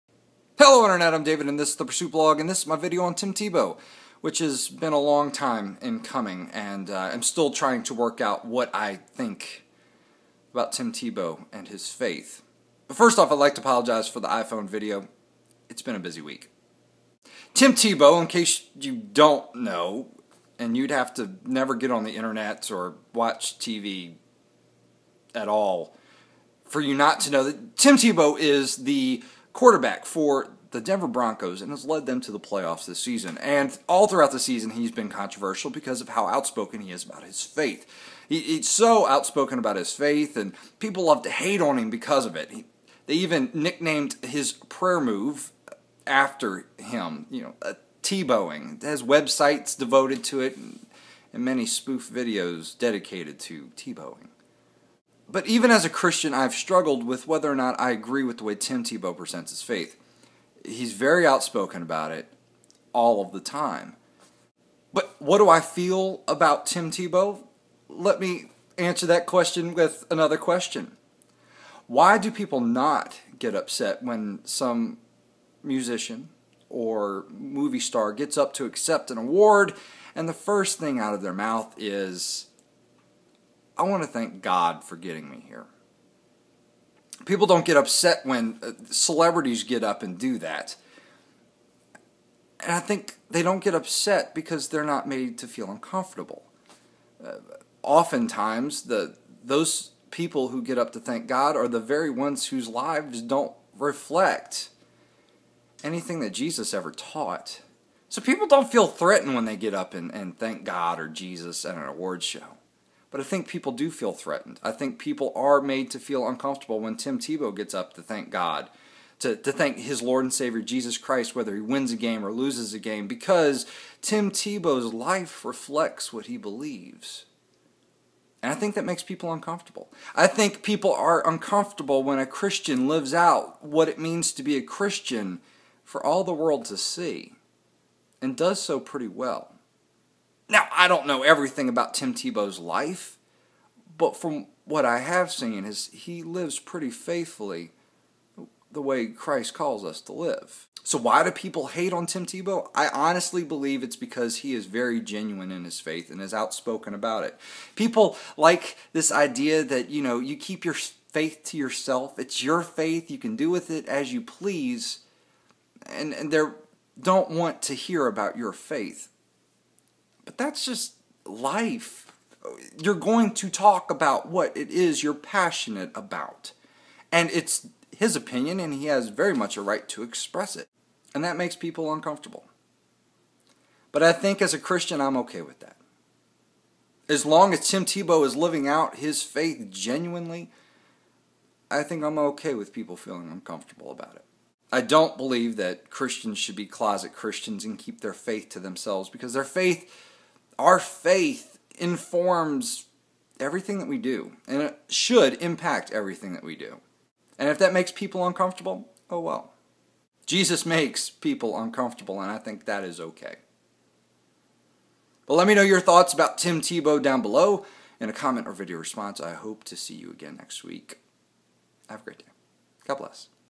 Why People HATE Tim Tebow
Tim Tebow is a galvanizing person. You either love him or hate him. And I believe people hate him because his faith makes them uncomfortable...

INTERACT with me Here!